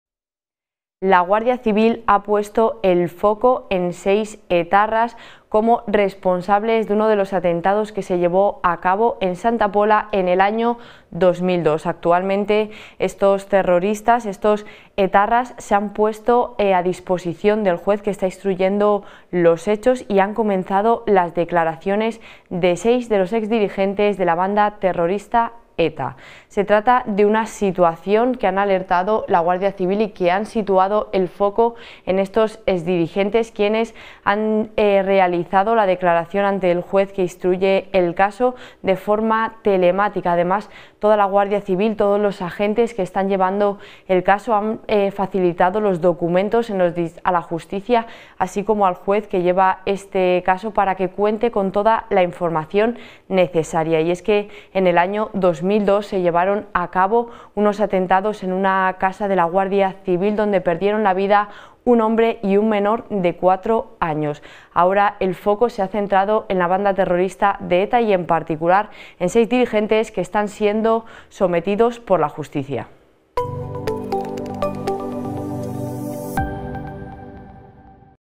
La Guardia Civil apunta a 6 exjefes de ETA como responsables del atentado de Santa Pola
La Guardia Civil sostiene que seis integrantes de la cúpula de ETA ordenaron el atentado contra la casa cuartel de la Guardia Civil de Santa Pola (Alicante), en el que murió un hombre y una niña de seis años, el 4 de agosto de 2002, y apunta a que pudo ser en respuesta al proceso de ilegalización de Batasuna. Estas son las principales conclusiones del informe de 312 folios que la Guardia Civil ha remitido al juez de la Audiencia Nacional Manuel García Castellón, que en julio acordó citar como investigados a estos seis exdirigentes etarras tras admitir a trámite una querella de la asociación Dignidad y Justicia. Los seis -Antonio Olarra Guridi, alias Juanvi, Jon, Jokin y Fede; Ainhoa Mugica Goñi, Olga; Félix Ignacio Esparza Luri; Ramón Sagarzazu Gaztelumendi, alias Ramontxo o Txango; María Soledad Iparraguirre Guenechea, Marisol o Anboto, y Mikel Albisu, Mikel Antza- prestan declaración este lunes por videoconferencia ante el juez por estos hechos